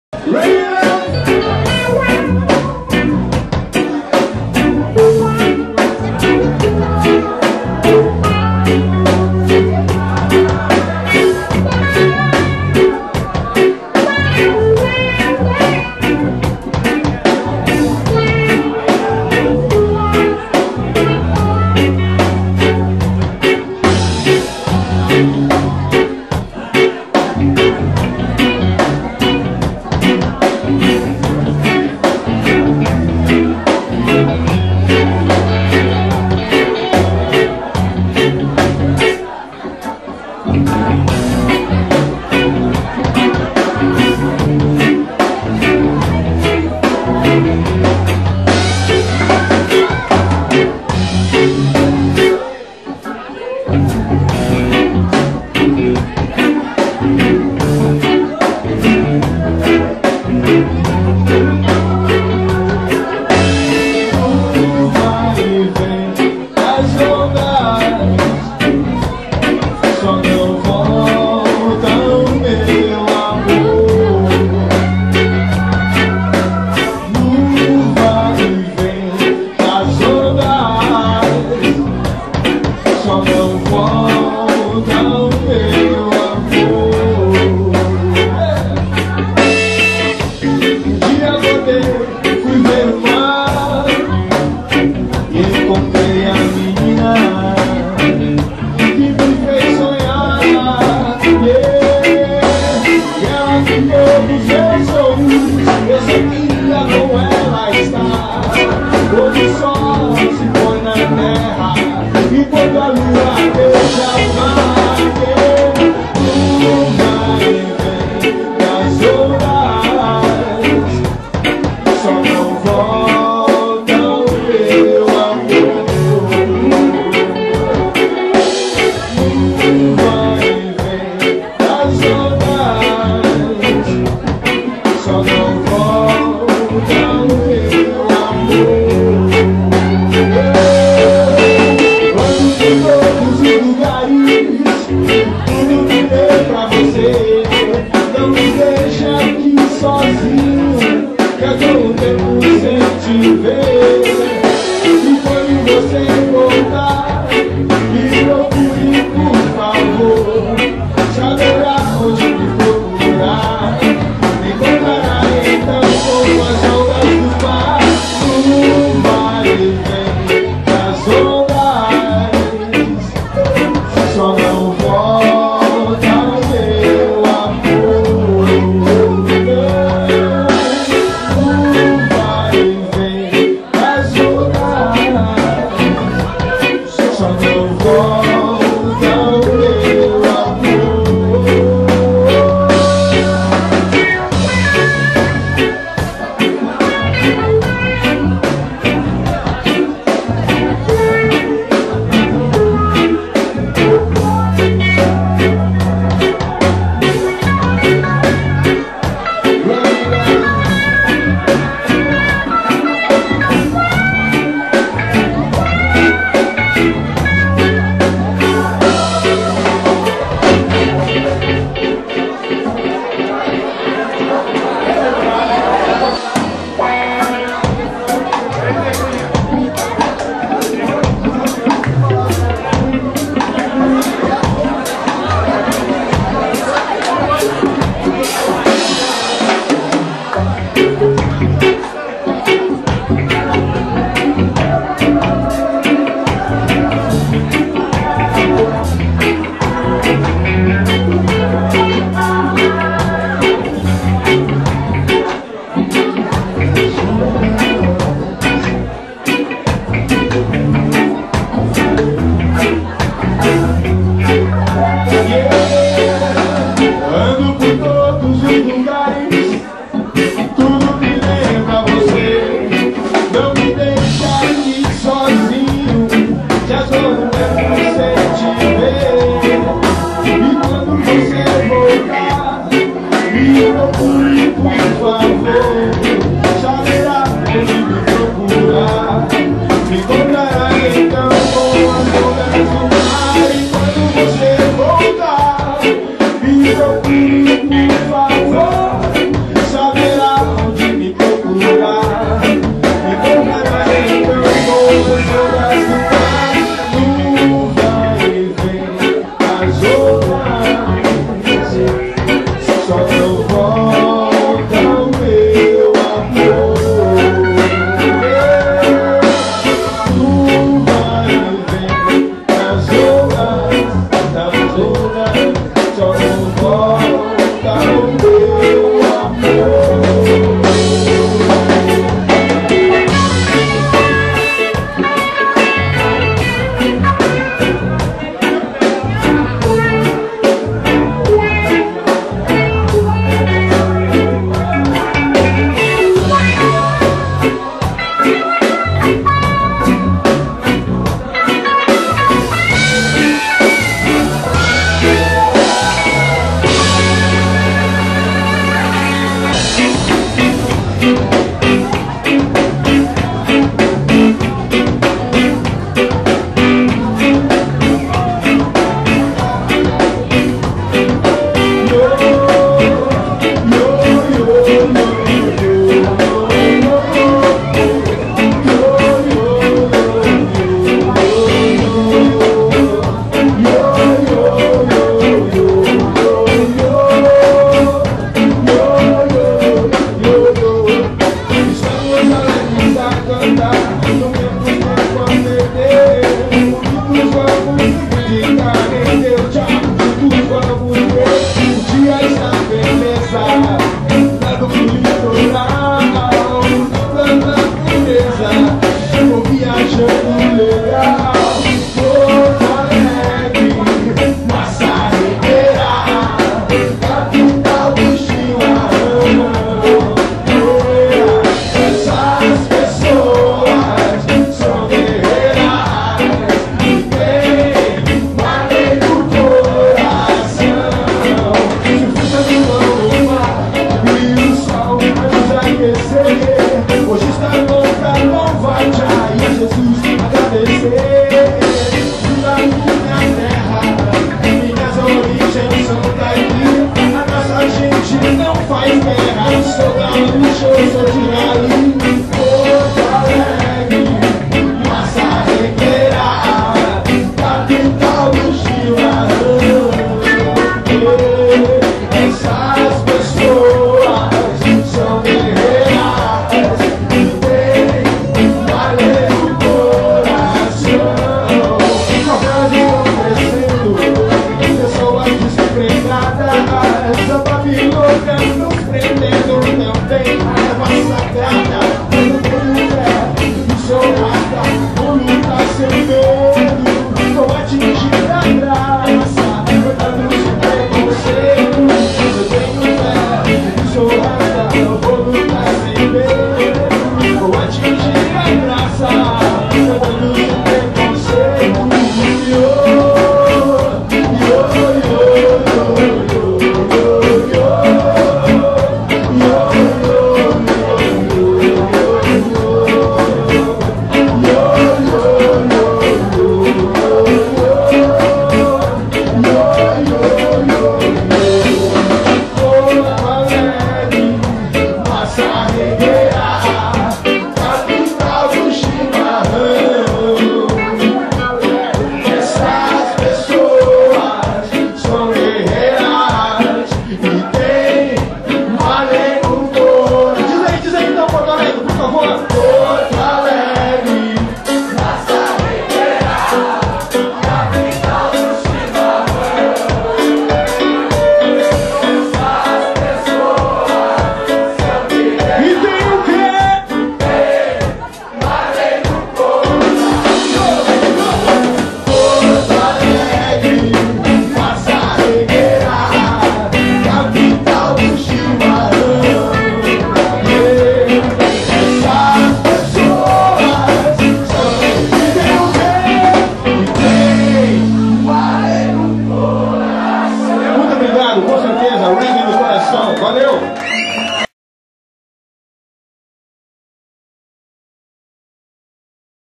Planet Roots - Parte 3
A Radio Pirada, através da locutora Moema, entrevistou a banda Planet Roots, e registrou todo show da banda no dia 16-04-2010, no bar Bate Macumba, em Porto Alegre - RS.